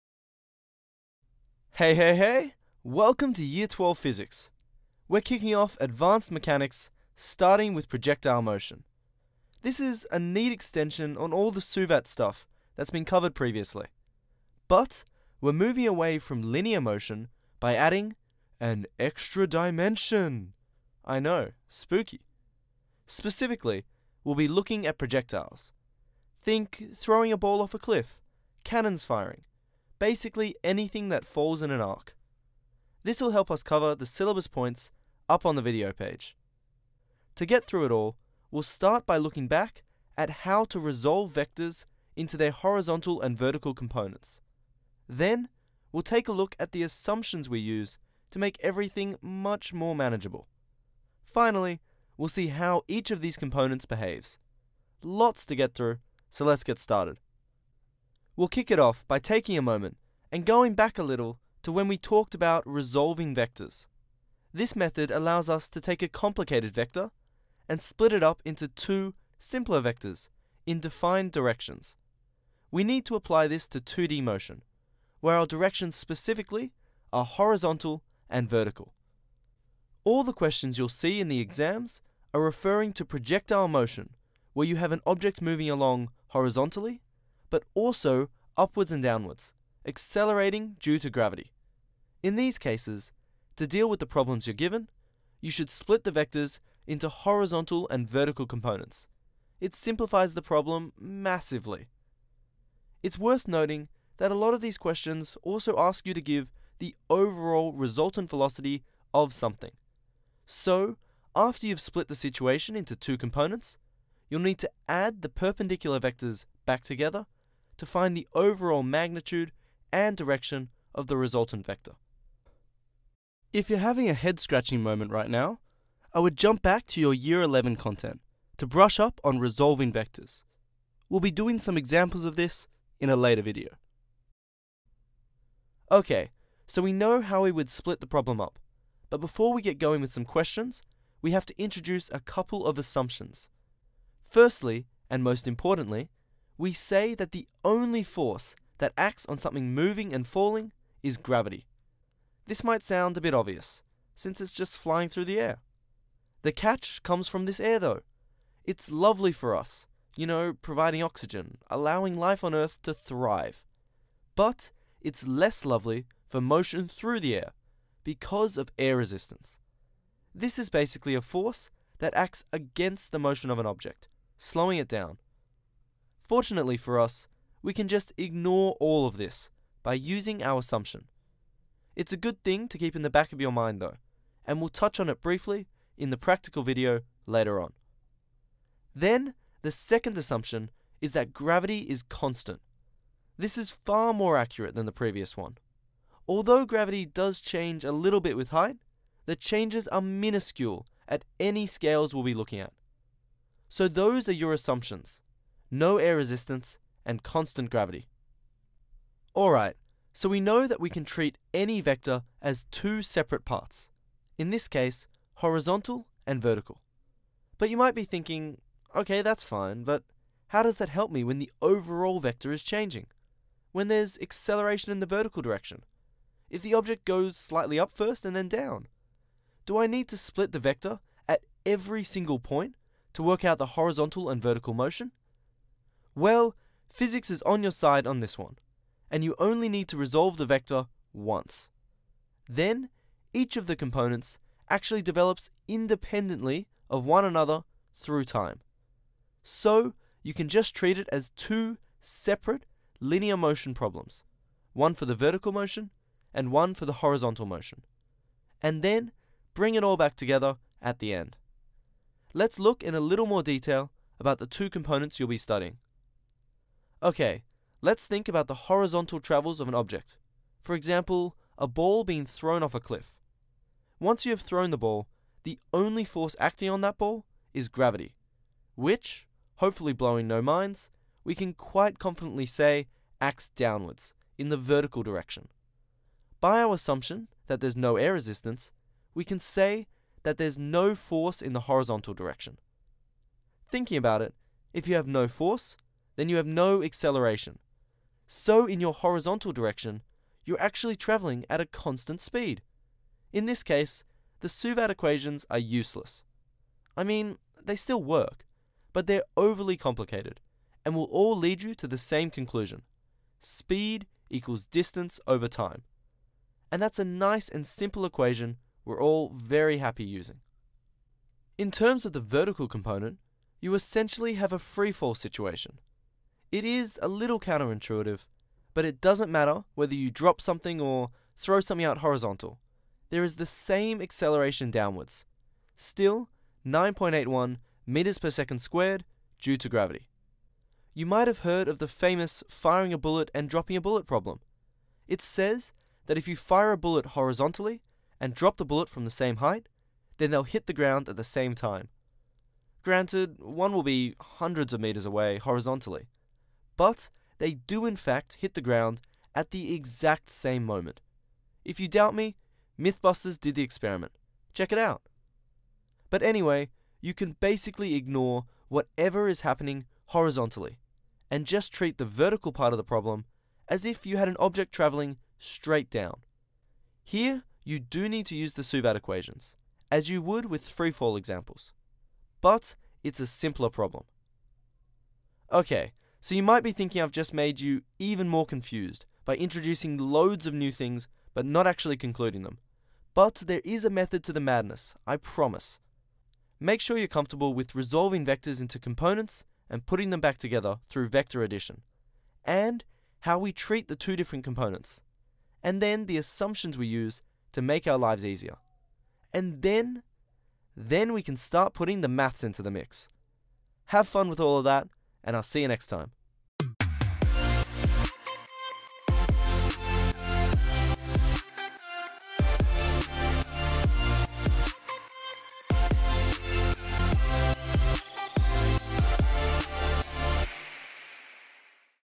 Y11-12 Physics: 2D Motion
In this HSC Physics video, we look at two-dimensional motion and see how we can use of knowledge of linear motion in this novel situation.

NSW, QLD, VIC, SA, WA Atar,